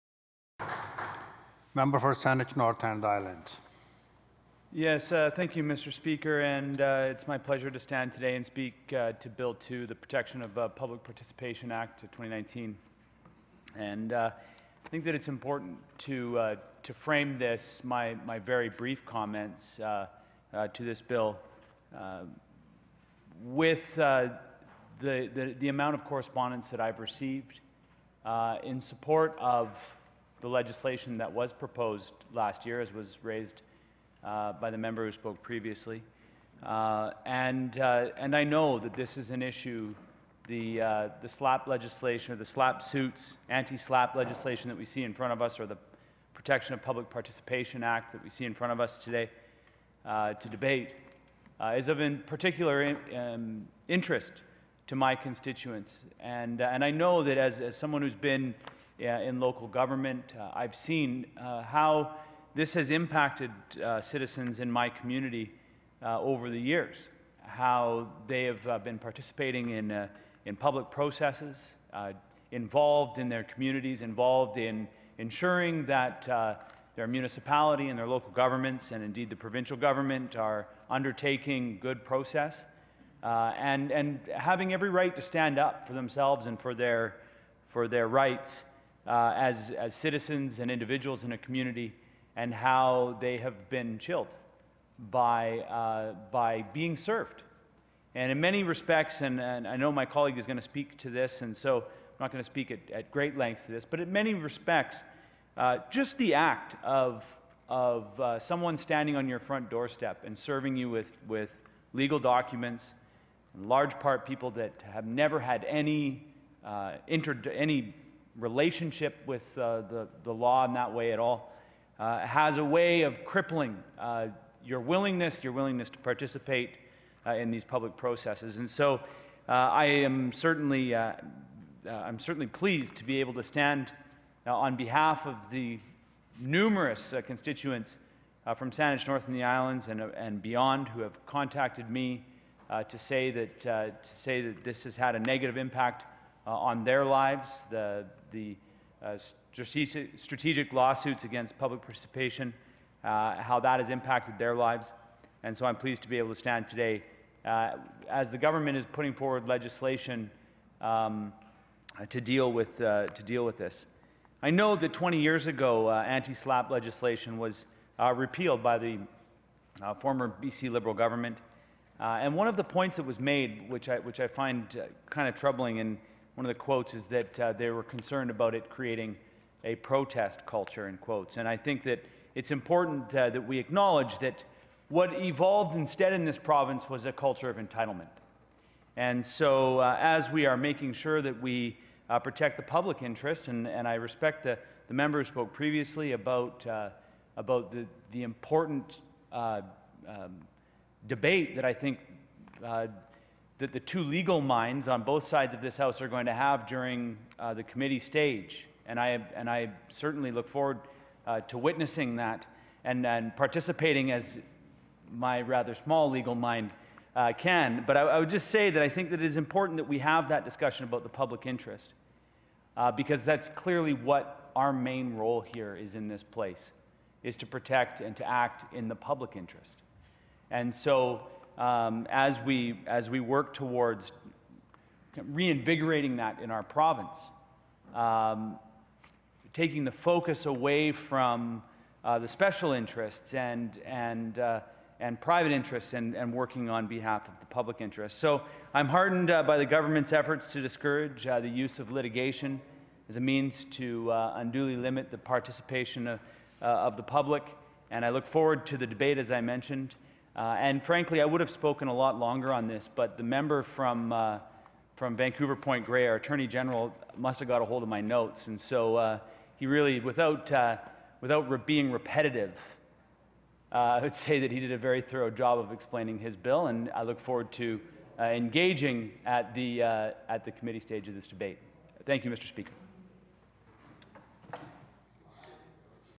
Anti-SLAPP legislation debated in British Columbia
We finally get to debate anti-SLAPP legislation in British Columbia. Many constituents of Saanich North and the Islands have written my office encouraging us to bring this legislation in to force and I am happy it is now in front of us.

[Transcript]

It's my pleasure to stand today and speak to Bill 2, the Protection of Public Participation Act of 2019. I think that it's important to frame my very brief comments on this bill with the amount of correspondence that I have received in support of the legislation that was proposed last year, as was raised by the member who spoke previously.
I know that this is an issue — the SLAPP suits, anti-SLAPP legislation or the Protection of Public Participation Act that we see in front of us today to debate — that is of particular interest to my constituents. I know that as someone who's been in local government, I've seen how this has impacted citizens in my community over the years, how they have been participating in public processes, involved in their communities and involved in ensuring that their municipality and their local governments and, indeed, the provincial government are undertaking good process, and having every right to stand up for themselves and for their rights as citizens and individuals in a community and how they have been chilled by being served.
I know my colleague is going to speak to this, and so I'm not going to speak at great length to this. But in many respects, just the act of someone standing on your front doorstep and serving you with legal documents — and in large part, people that have never had any entry to any relationship with the law in that way at all — has a way of crippling your willingness and your willingness to participate in these public processes.
So I'm certainly pleased to be able to stand on behalf of the numerous constituents from Saanich North and the Islands and beyond who have contacted me to say that this has had a negative impact on their lives — the strategic lawsuits against public participation and how that has impacted their lives. I'm pleased to be able to stand today as the government is putting forward legislation to deal with this.
I know that 20 years ago, anti-SLAPP legislation was repealed by the former B.C. Liberal government. One of the points that was made, which I find kind of troubling in one of the quotes, is that they were concerned about it creating a "protest culture." I think that it's important that we acknowledge that what evolved instead in this province was a culture of entitlement.
So as we are making sure that we protect the public interest, and I respect the member who spoke previously about the important debate that I think that the two legal minds on both sides of this House are going to have during the committee stage. I certainly look forward to witnessing that and participating as my rather small legal mind can.
But I would just say that I think that it is important that we have that discussion about the public interest because that's clearly what our main role here is in this place, is to protect and to act in the public interest.
So as we work towards reinvigorating that in our province, taking the focus away from the special interest and private interests and working on behalf of the public interests, I'm heartened by the government's efforts to discourage the use of litigation as a means to unduly limit the participation of the public. I look forward to the debate, as I mentioned.
Frankly, I would have spoken a lot longer on this, but the member for Vancouver–Point Grey, our Attorney General, must have got hold of my notes, so he really…. Without being repetitive, I would say that he did a very thorough job of explaining his bill, and I look forward to engaging at the committee stage of this debate.